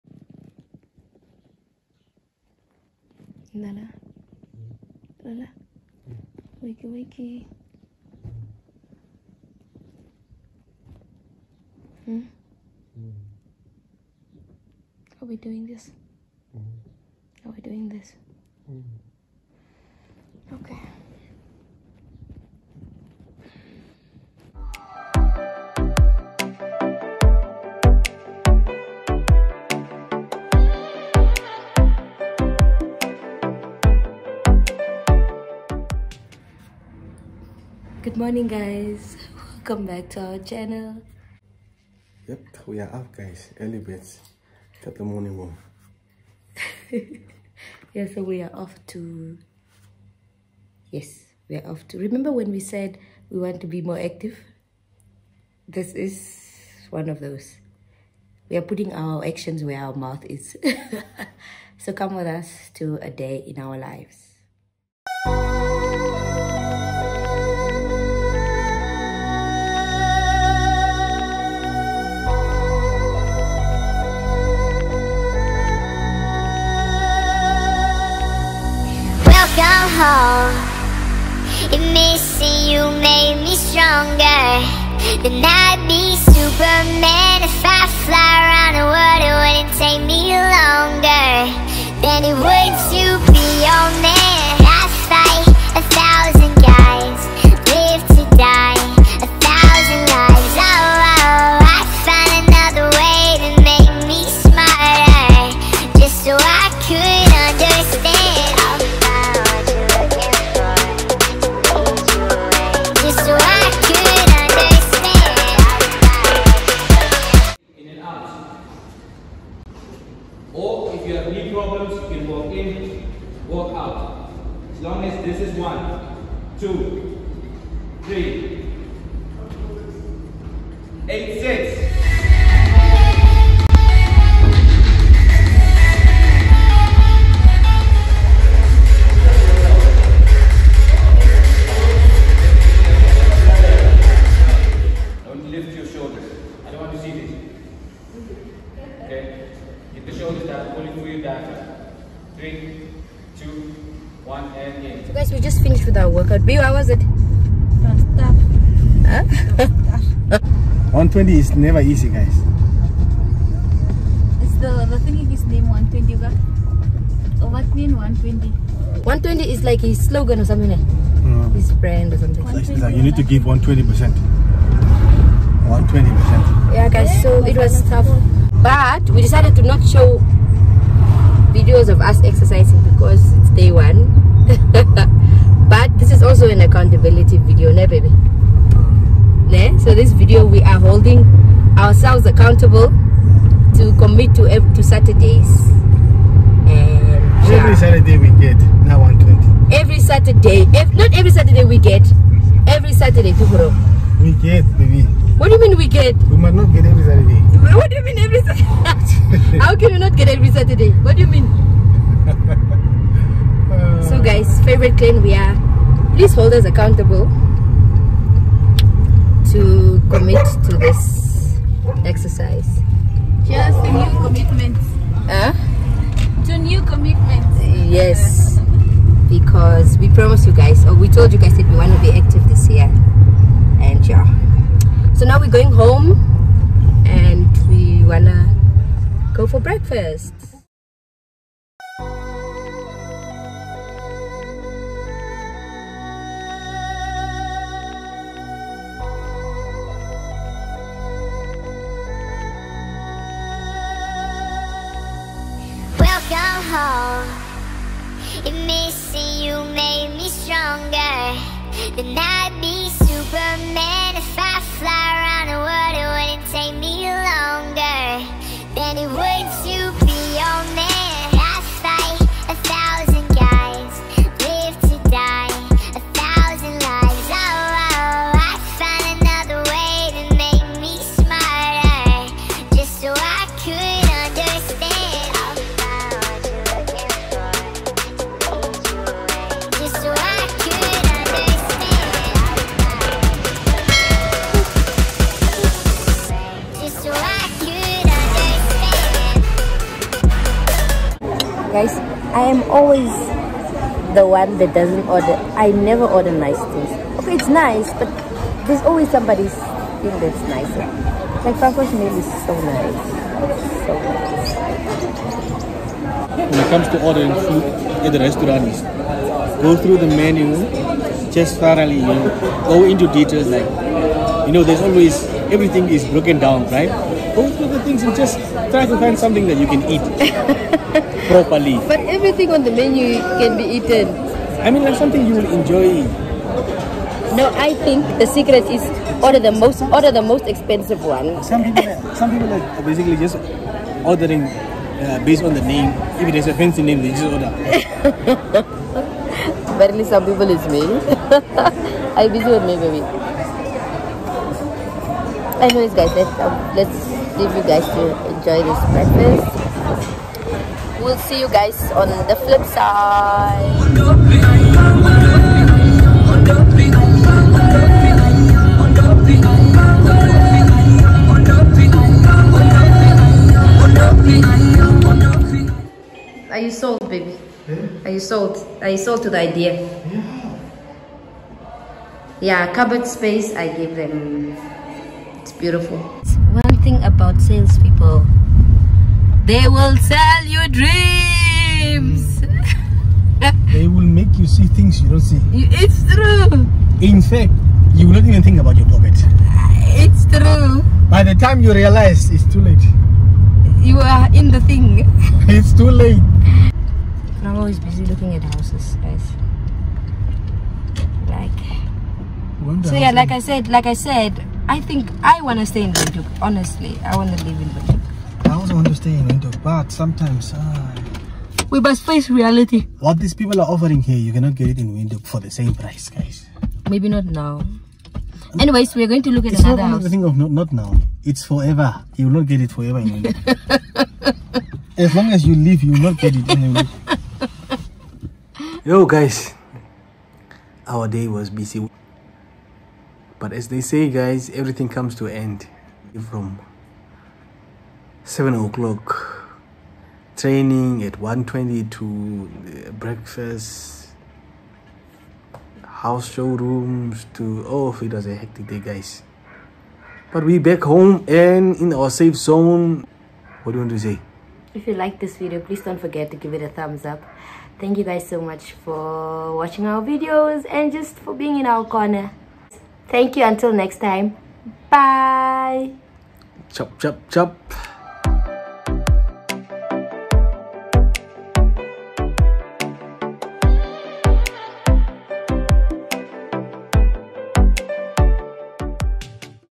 A Day in our Lives|We Almost Bought a House We Cannot Afford|Gym Commitments|Getting Breakfast
Halo Fav Clan

Enjoy this very short episode of what we got up to over the weekend. We know y’all want longer videos. We working on that🙈

Xoxoxoxoxo

The Aoxamubs